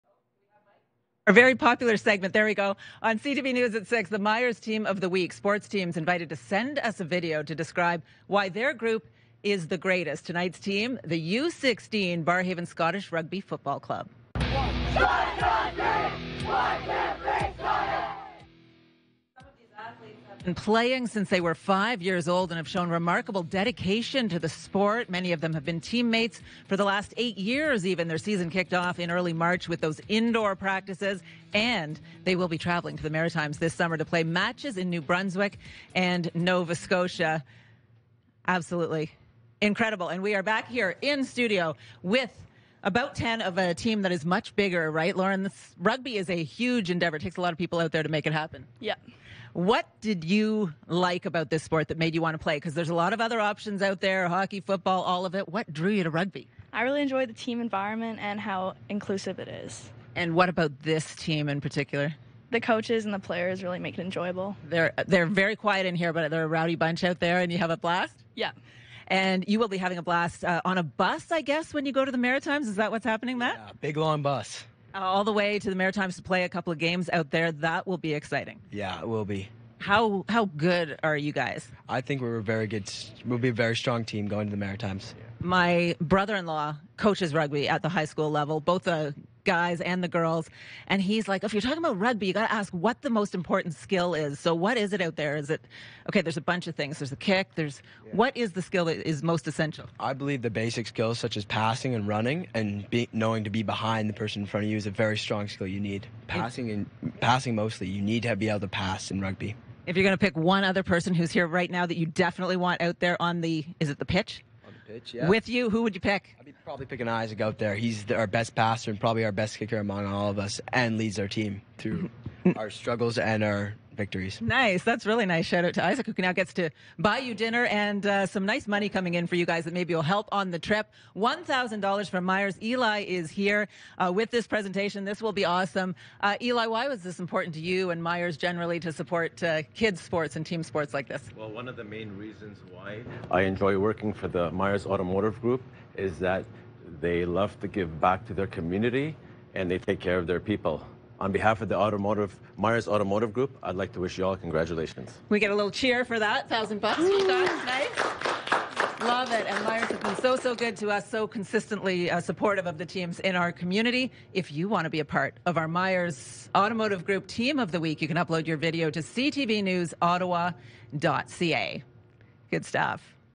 Myers Team of the Week | Week 2 | U16 Barrhaven Scottish RFC
Congratulations to the U16 Barrhaven Scottish RFC 🏉🎉

They are this week's Myers Team of the Week!

Sales Manager Elie Aboumoussa from Barrhaven Hyundai was more than happy to present the cheque 🥰